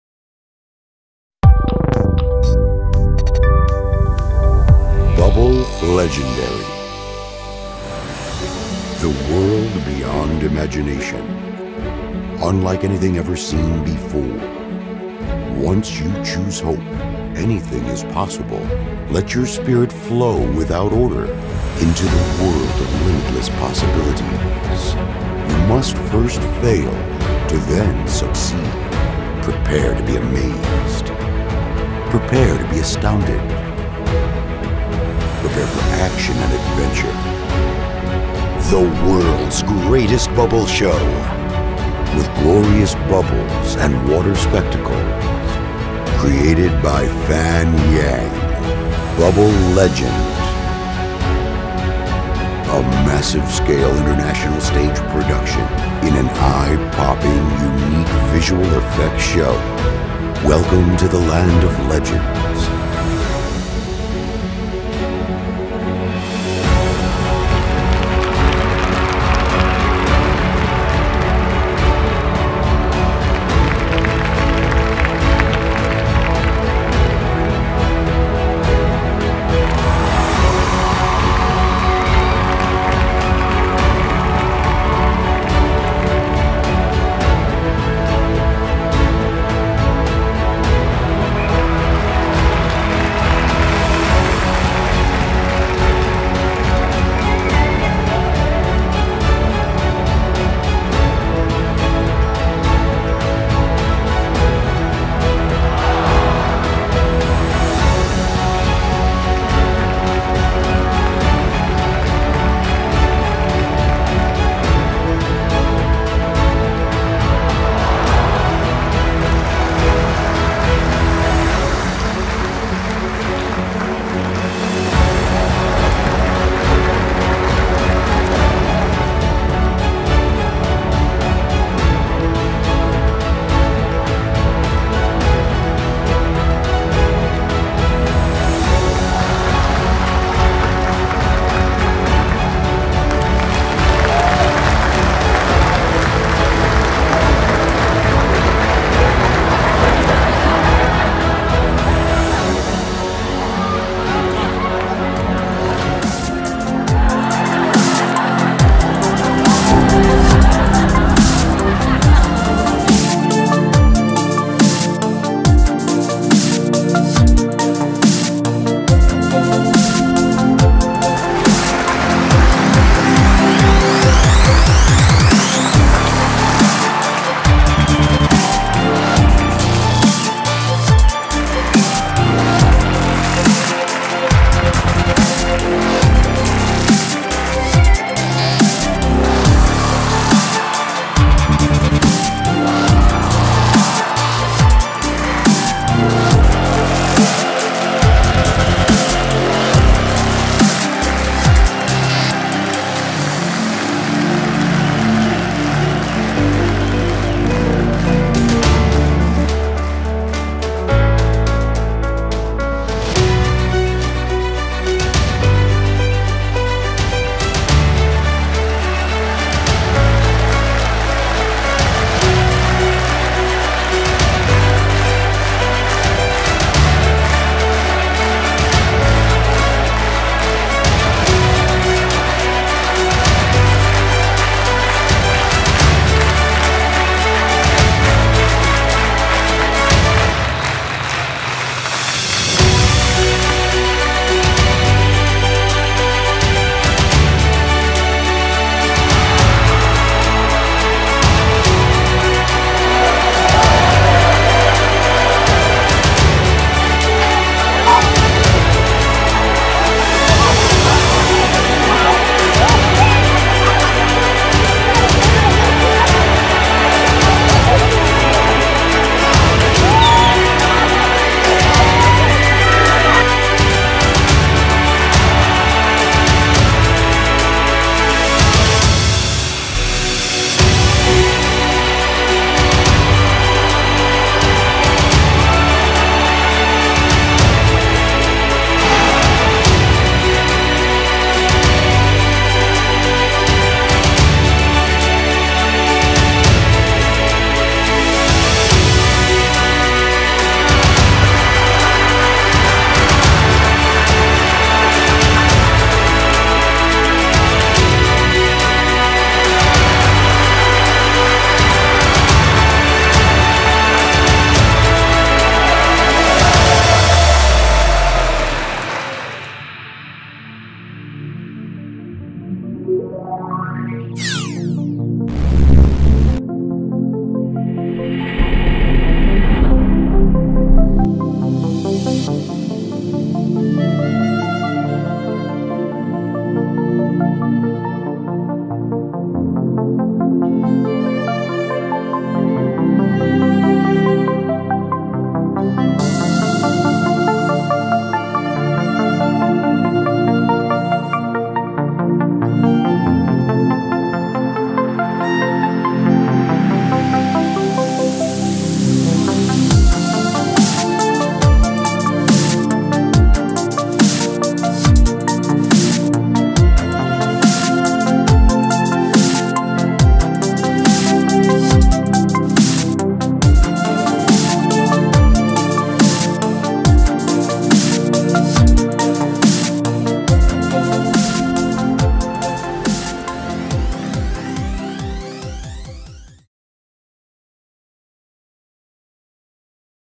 BL Trailer
You are watching feature presentation of The World's Greatest Bubble Show. A massive international stage production
never seen before. "Bubble Legendary" created by Fan Yang
Holder of 19 Guinness World Records and creator of Gazillion Bubble Show